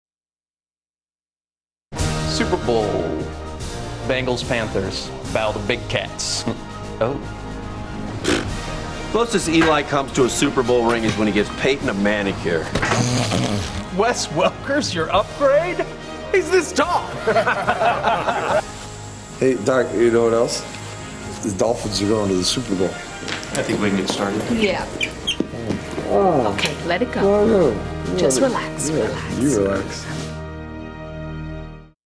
NFL Network Super Bowl Ad - Bold Predictions
A series of scenes that show fans before the season making statements that the season proves to be wrong.